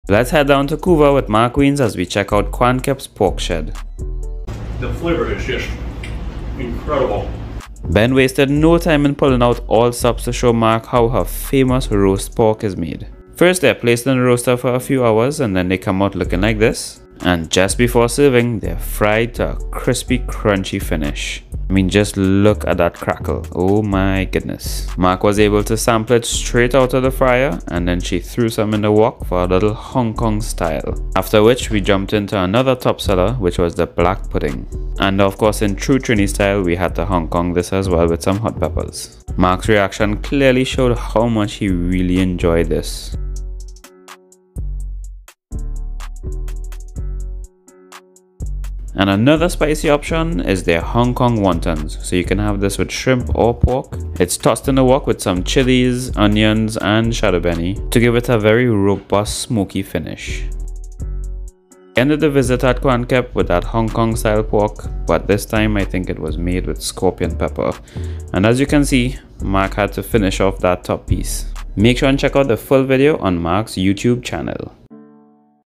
Quan Kep's Pork Shed with Mark Wiens
A stop at Quan Kep's Pork Shed isn't complete without trying their famous roast pork, black pudding and wontons.